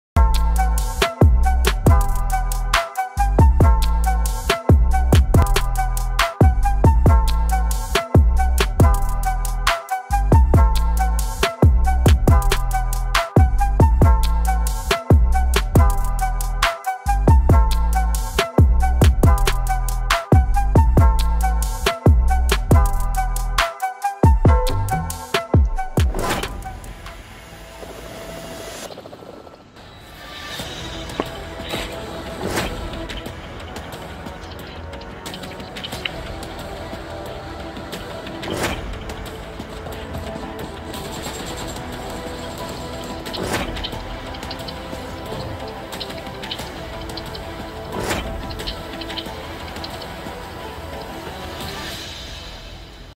PUBG Mobile 0.15.5 launches, updates new jungle maps, new characters, new guns
Players who install the 0.15.5 update before November 14 will receive many attractive gifts and 20 silver coins, 2,000 BP.